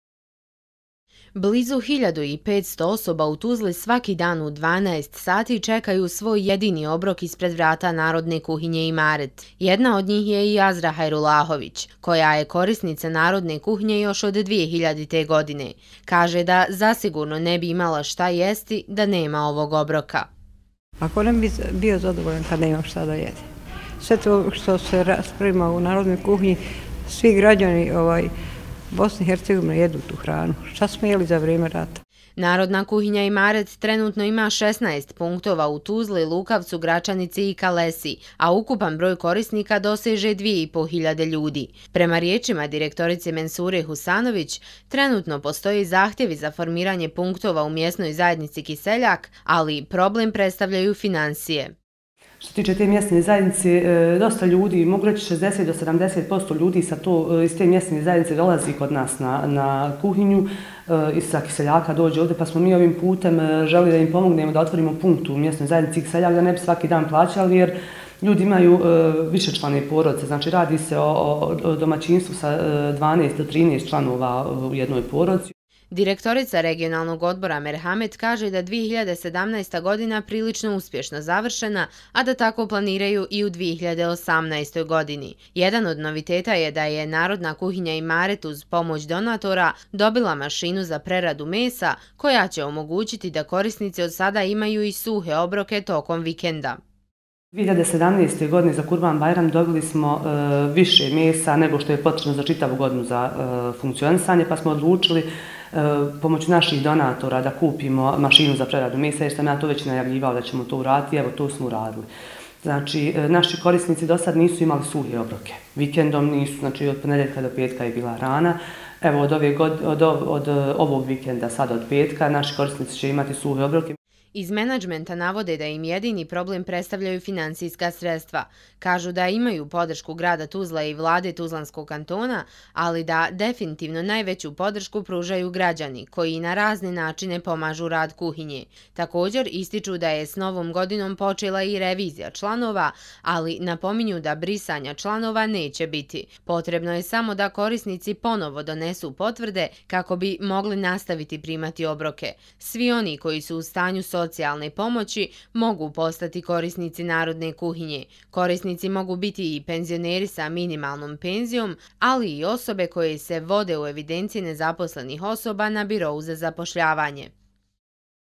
Narodna kuhinja „Imaret“: Korisnici će sada imati i suhe obroke - 22.01.2018.
Na području Tuzlanskog kantona sve više je osoba koje su u stanju socijalne potrebe. Jedan obrok dnevno sebi ne mogu priuštiti, te se za pomoć obraćaju Narodnoj kuhinji Imaret u Tuzli. Međutim i Narodna kuhinja sve teže odgovara njihovim zahtjevima zbog loše finansijske situacije.